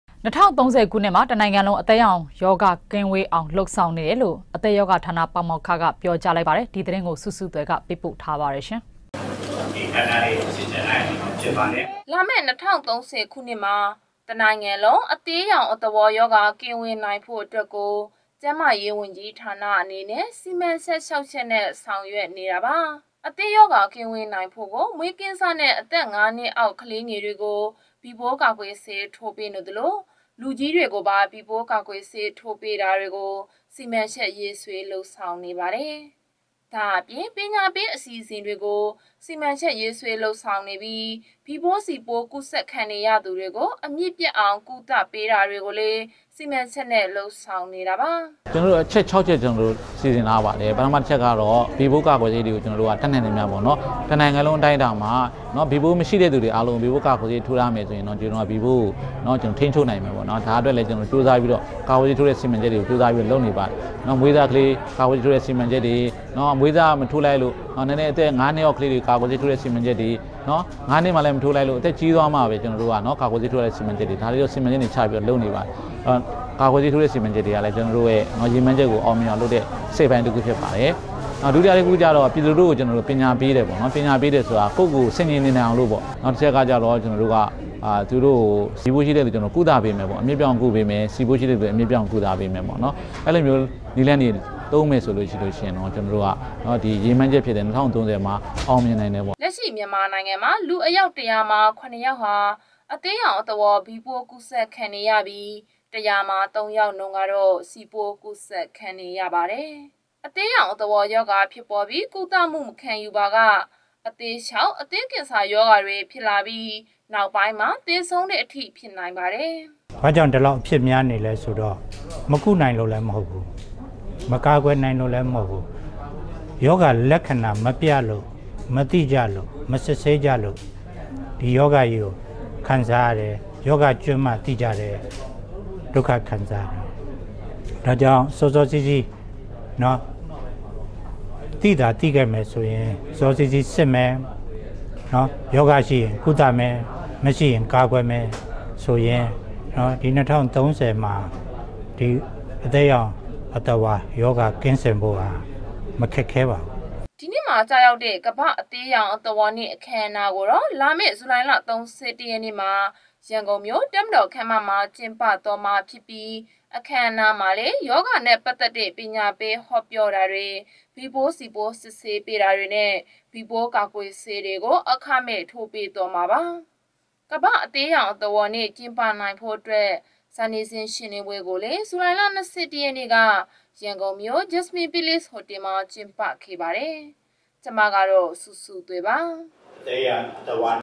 DVB - (၂၀၃၀) ခုနွစ္မွာ တစ္နုိင္ငံလံုး အသည္းေရာင္ေရာဂါကင္းေဝးေအာင္ လုပ္ေဆာင္ေန
(၂၀၃၀) ခုနွစ္မွာ တစ္နုိင္ငံလံုး အသည္းေရာင္ေရာဂါကင္းေဝးေအာင္ လုပ္ေဆာင္ေနတယ္လုိ႔ အသည္းေရာဂါဌာန ပါေမာကၡက ေျပာပါတယ္။ ဒီသတင္း ကိုုဆုဆုသြယ္ကေပးပို႔ထားပါတယ္။